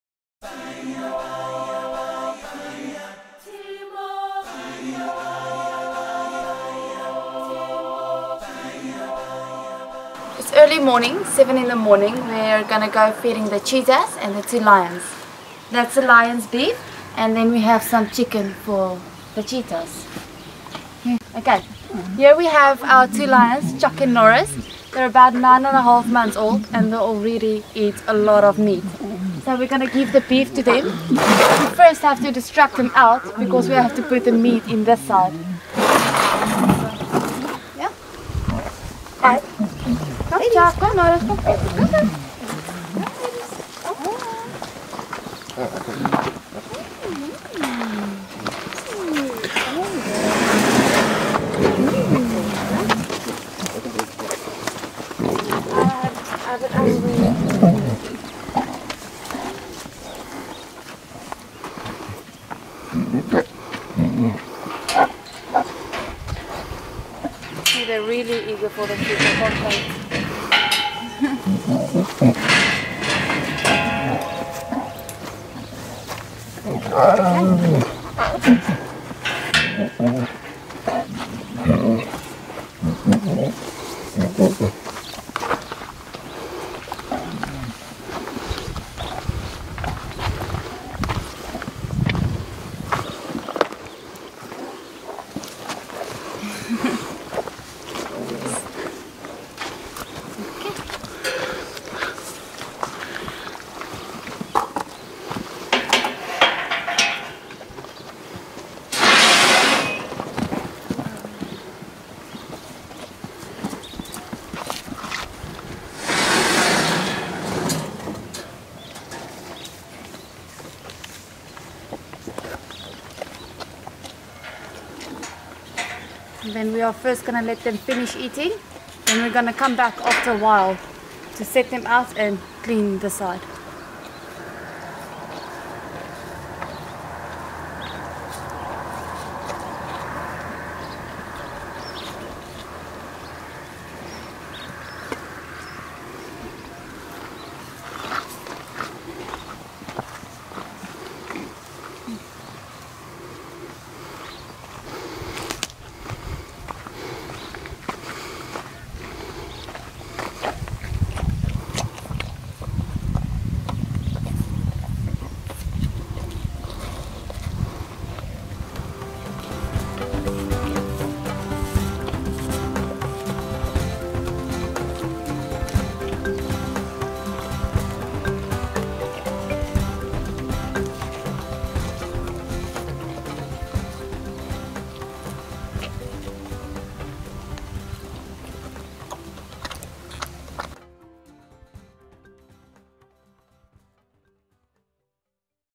Volunteers Feed the Lions at the Daniell Cheetah Project
Monique shows us how the volunteers at the Daniell Cheetah Project feed Chuck and Norris the Lions. Chuck and Norris are our ambassadors against canned hunting.